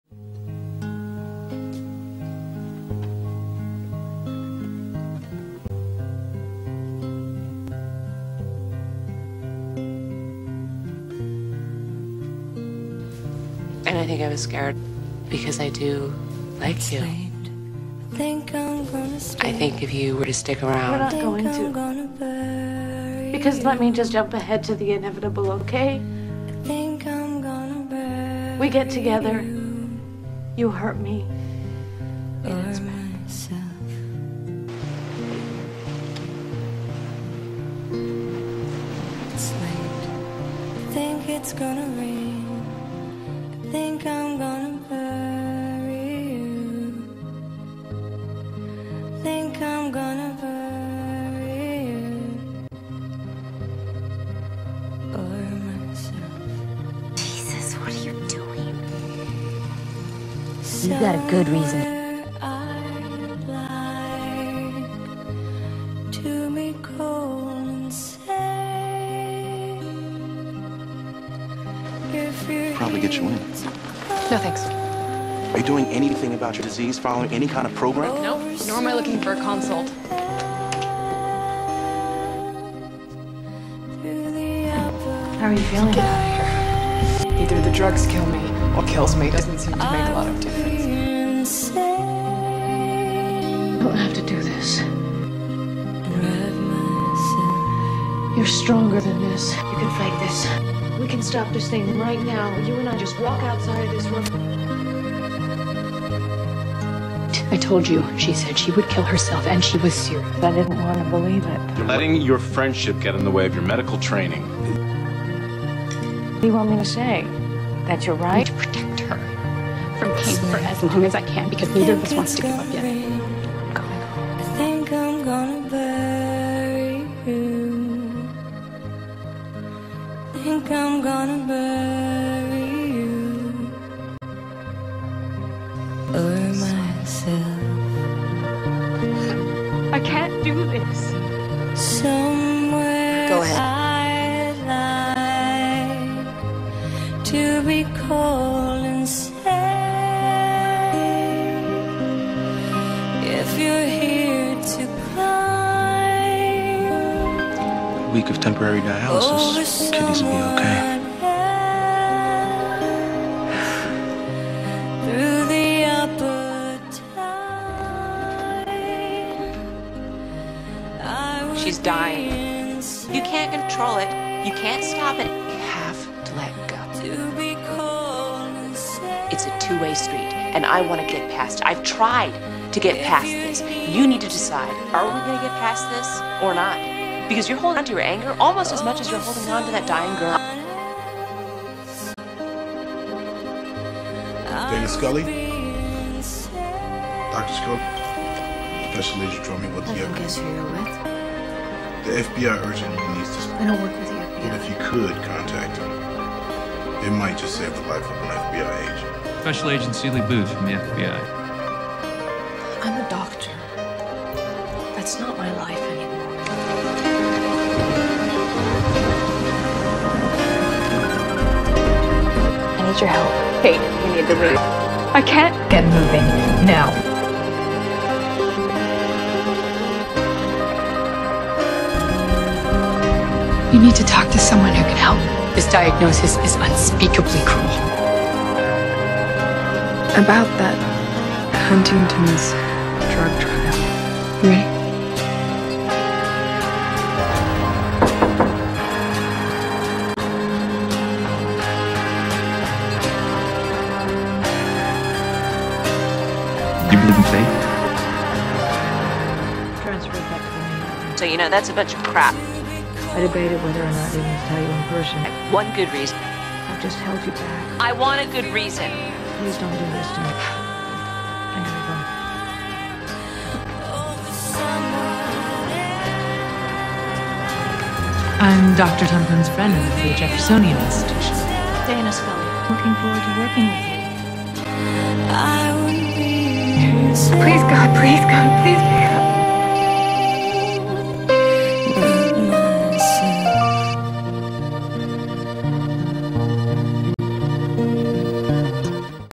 TKS: Chapter 6 - Bury You
Scully has a hard time dealing with Thirteen's illness and Kate falls deep into drugs and despair with Ana.